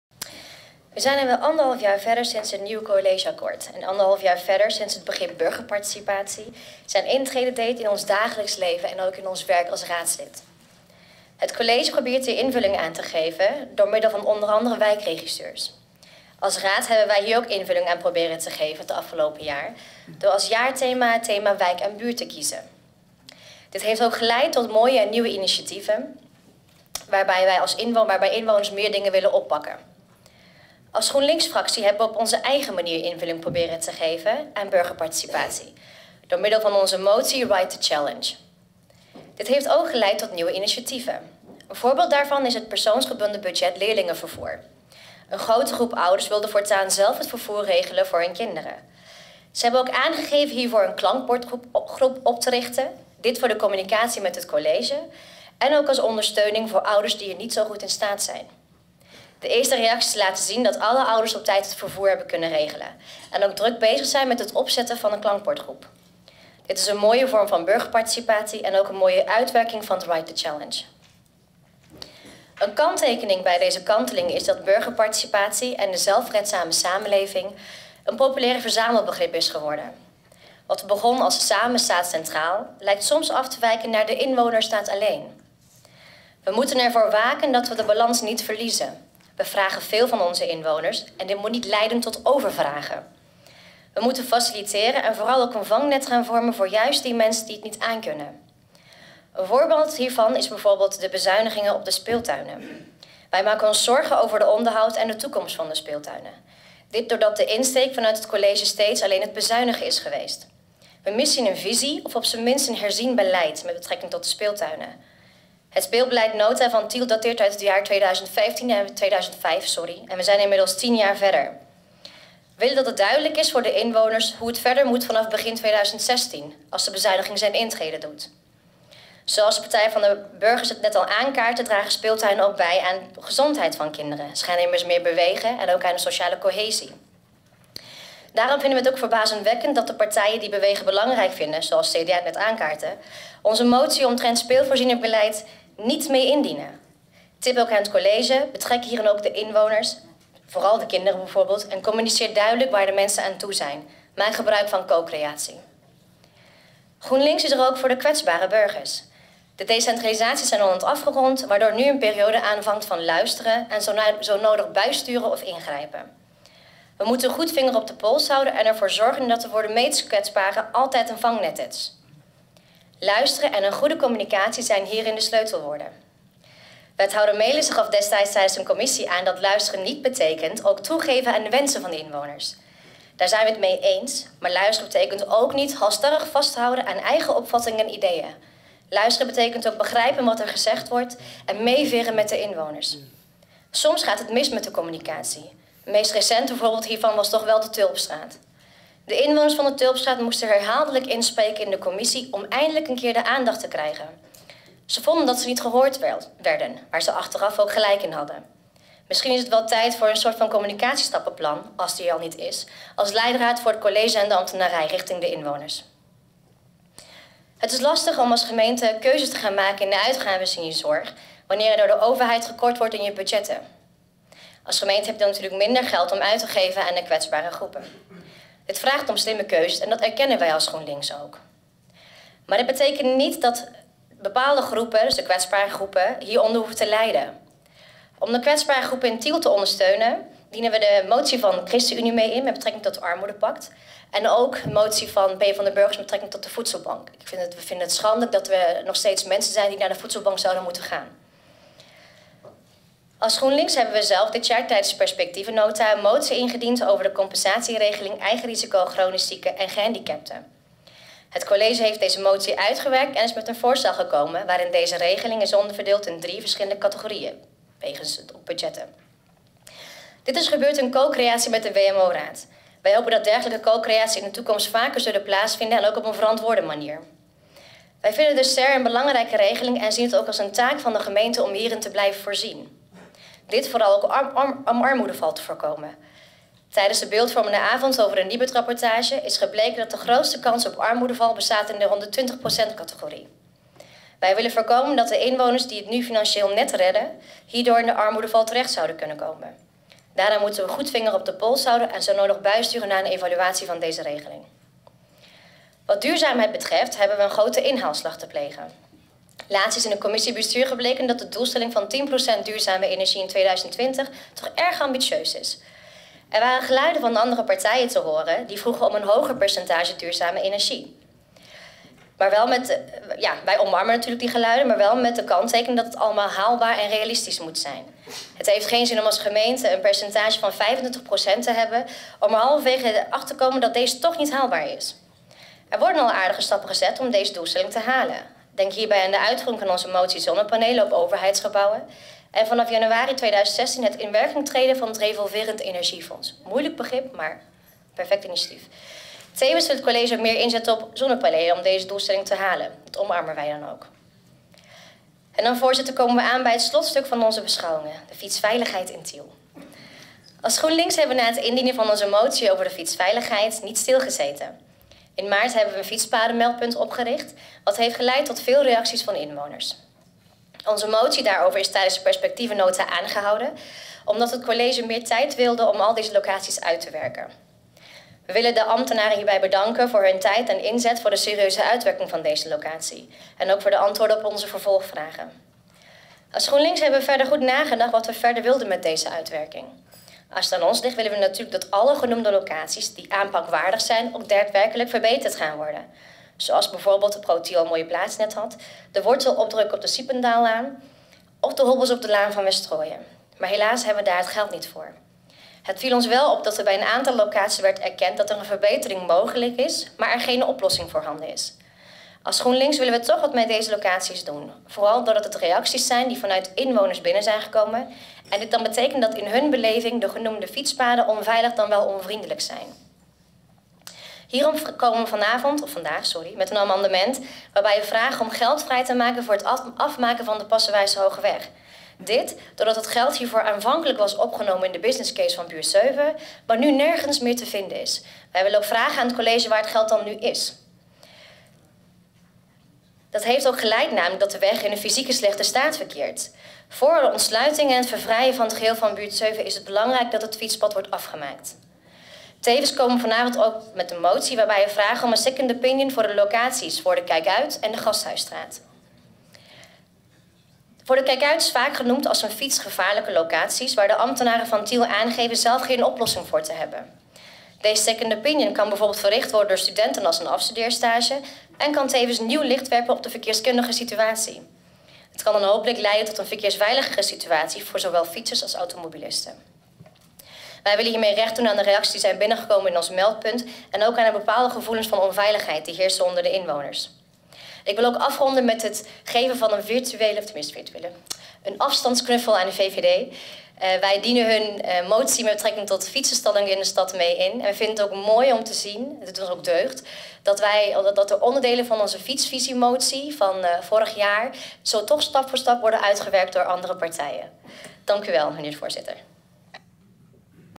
Begrotingsraad 11-11-2015: Beschouwing GroenLinks
Beschouwing op de begrotingsraad van fractievoorzitter GroenLinks, Armagan önder.